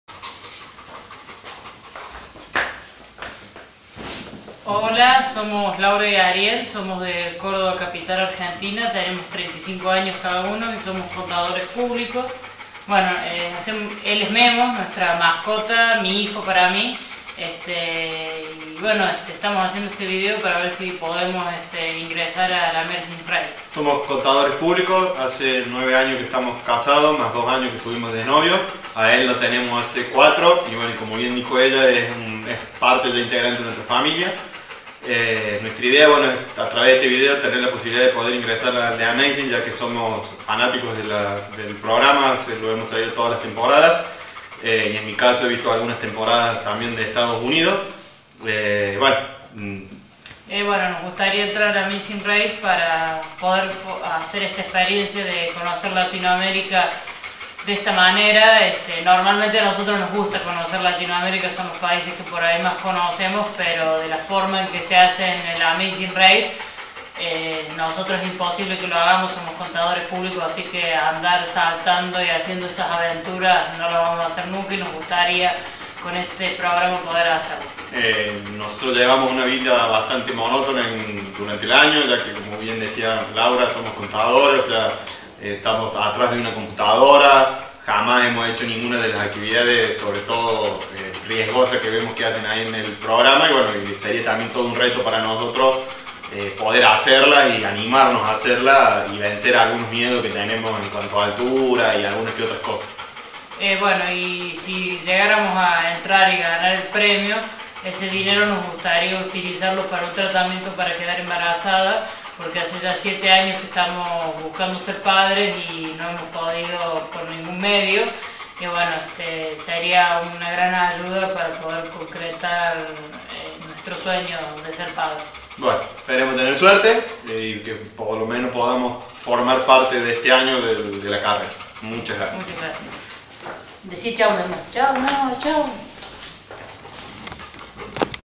Video presentación Laura y Ariel The Amazing Race Latinoamerica
Este es nuestro video presentación para el programa The Amazing Race Latinoamérica. Esperemos poder tener suerte este año!!